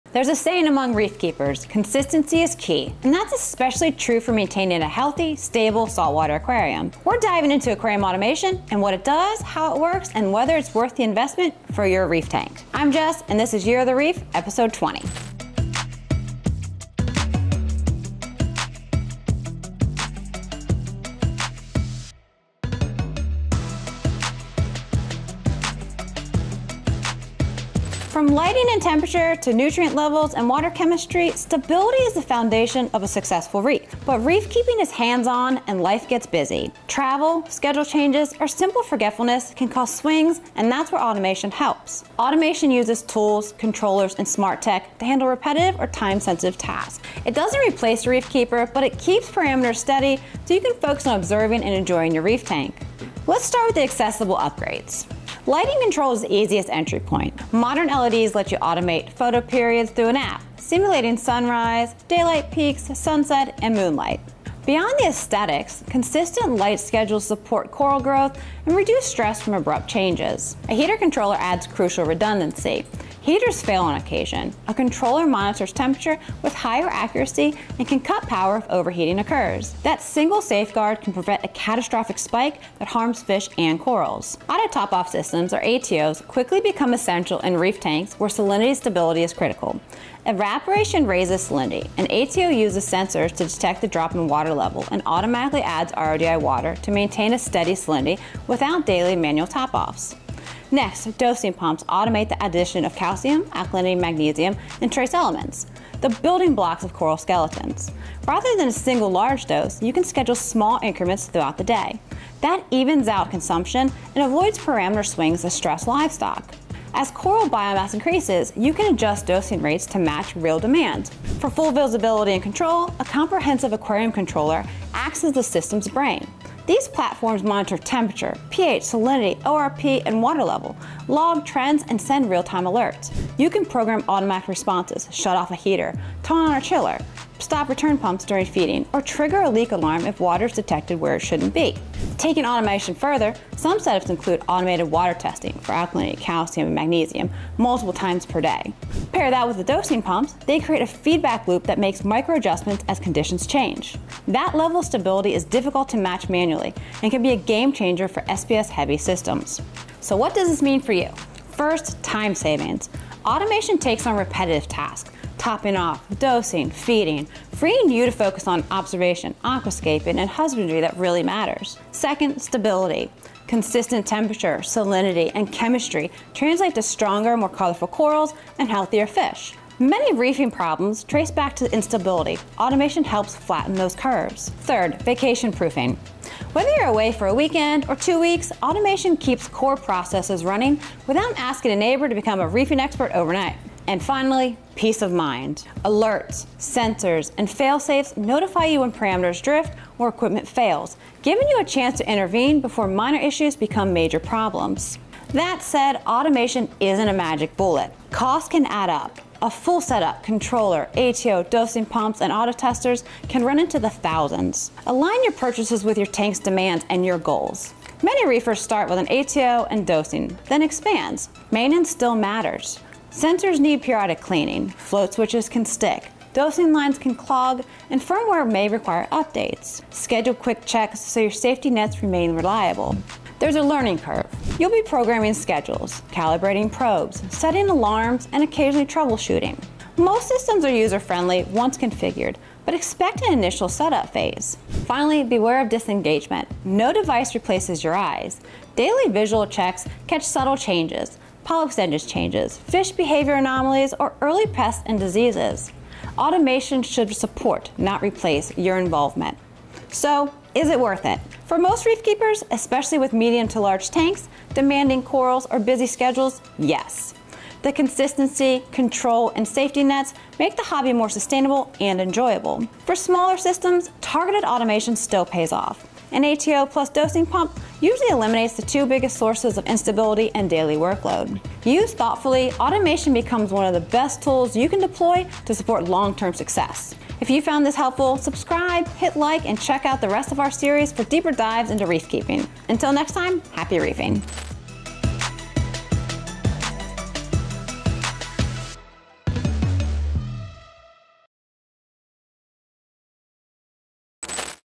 Aquarium Automation Explained | Year of the Reef Ep. 20
Consistency is the secret to a thriving reef tank — but life happens. Travel, busy schedules, or simple forgetfulness can throw your saltwater aquarium out of balance. That’s where aquarium automation steps in.

In this episode of Year of the Reef, Jess explores:
✅ The benefits of automation for lighting, temperature, salinity, and dosing
✅ Entry-level tools like ATOs, dosing pumps, and heater controllers
✅ Full aquarium controllers and auto-testing systems for advanced reefers
✅ The real costs, learning curve, and pitfalls you should know before diving in

We’ll answer the big question: Is automation worth the investment for your reef tank?

Whether you’re just starting with an auto top-off or considering a full controller system, this episode breaks down what actually makes a difference — and what you can skip.

💬 What’s your favorite piece of automation gear? Drop it in the comments!